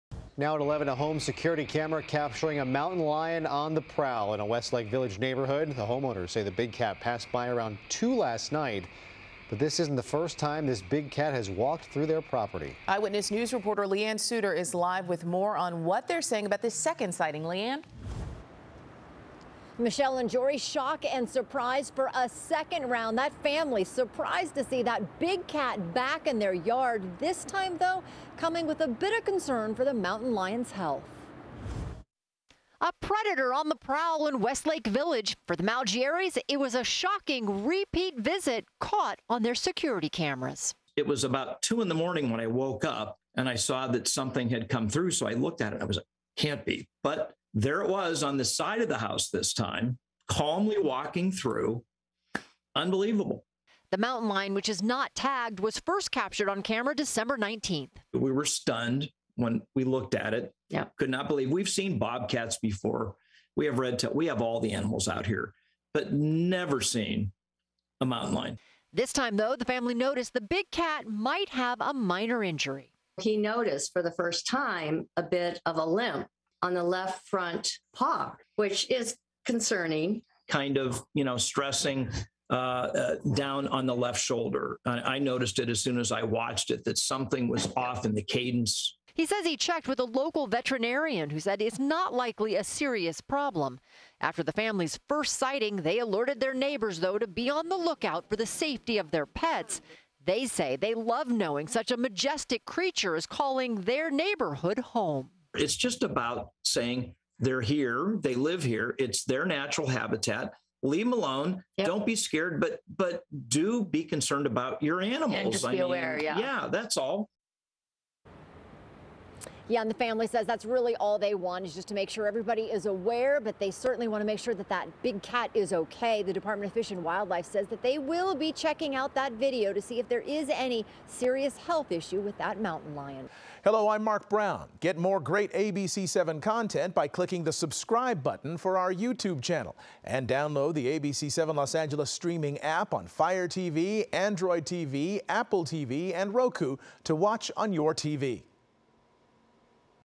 Video of mountain lion roaming SoCal neighborhood sparks excitement, health concerns
A home security camera captured a mountain lion prowling through a Westlake Village neighborhood, and the homeowners say it isn't the first time the big cat has walked through their property. MORE